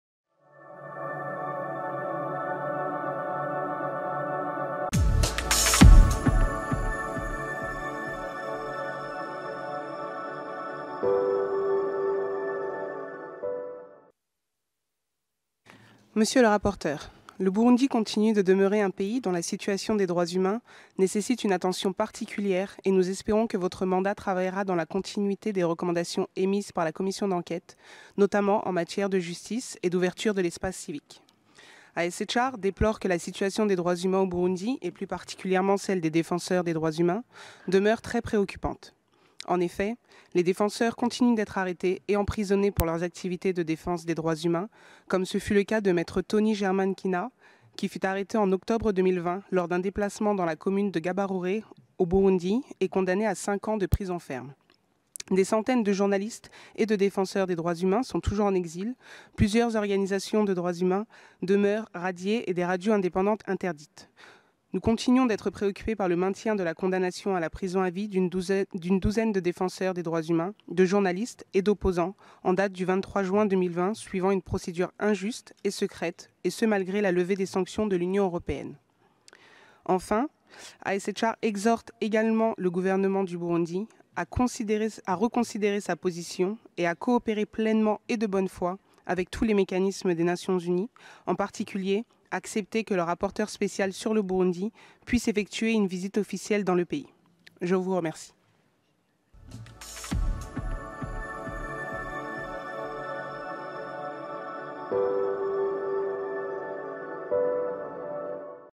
HRC50 ISHR calls on Government of Burundi to cooperate with the Special Rapporteur
ISHR delivered a statement highlighting how defenders continue to be arbitrary arrested and imprisoned, such as in the case of lawyer Tony Germain Nkina arrested in October 2020 in relation to his human rights work.